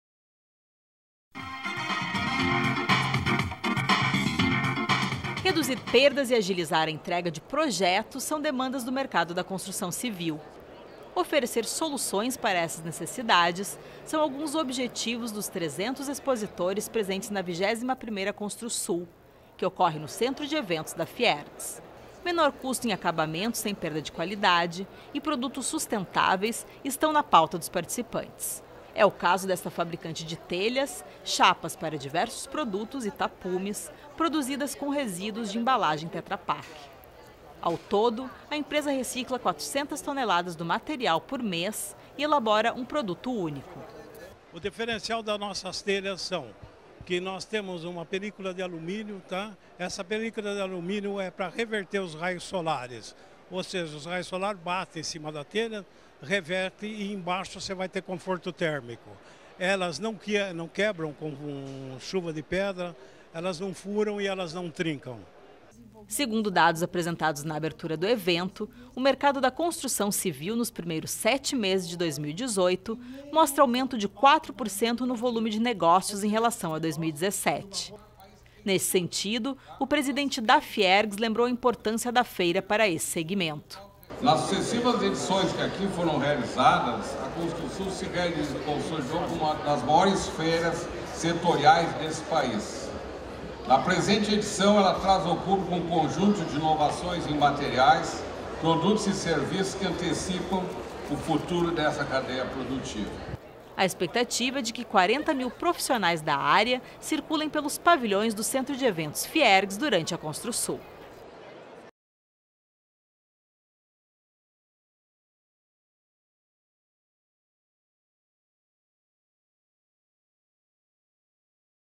Construsul apresenta tendências na construção civil
Com aproximadamente 300 expositores e a expectativa de atrair, até sábado (4), 40 mil profissionais dos setores de construção civil e arquitetura, a 21ª Construsul – Feira Internacional da Construção, ocorre no Centro de Eventos FIERGS.